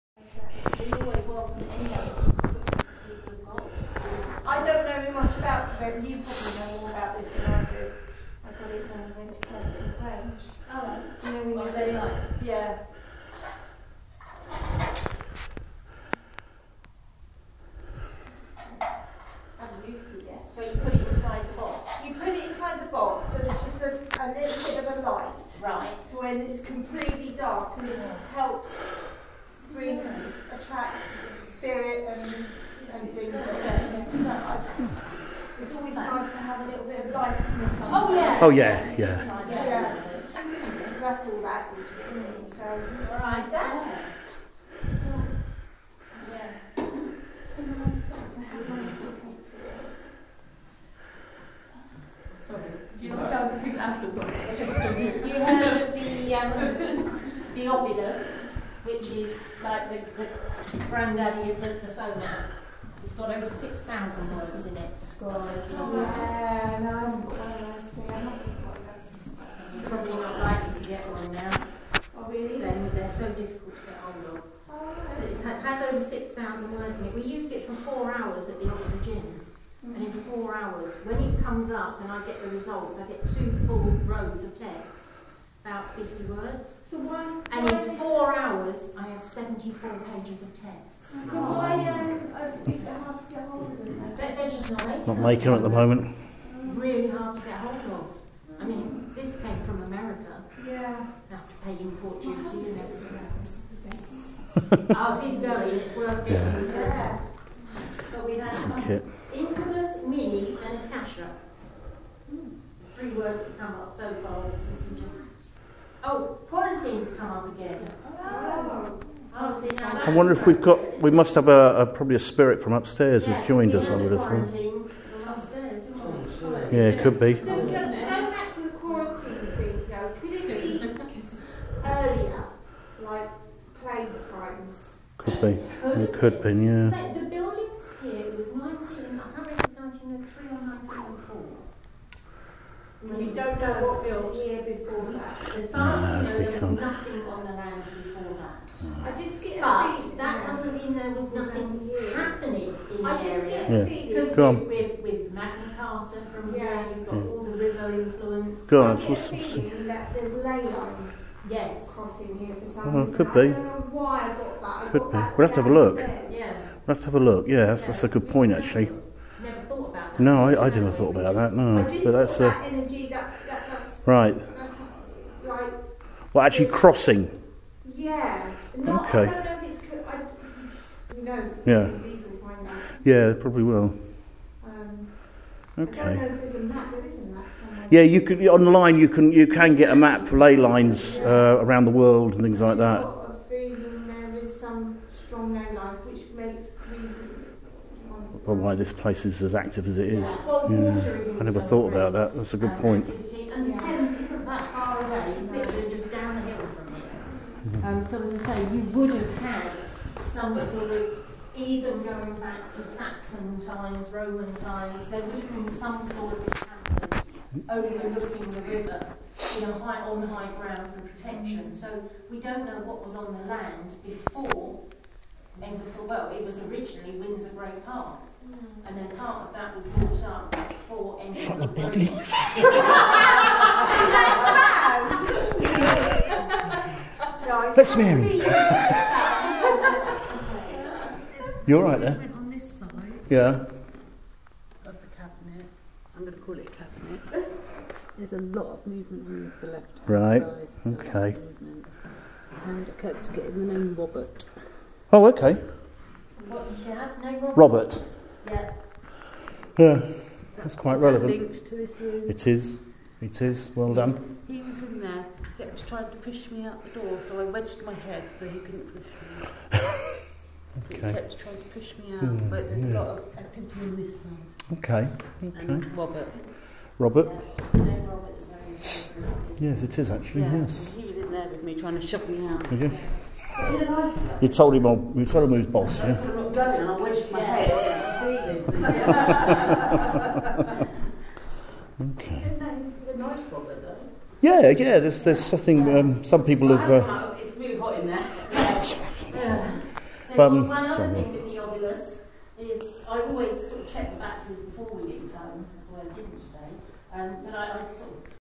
FRIGHT NIGHTS: BERKSHIRE THE OLD SOCIAL HALL, ENGLEFIELD GREEN GHOST HUNT - Sat 1st June 2019
Equipment and Discussion in Room 3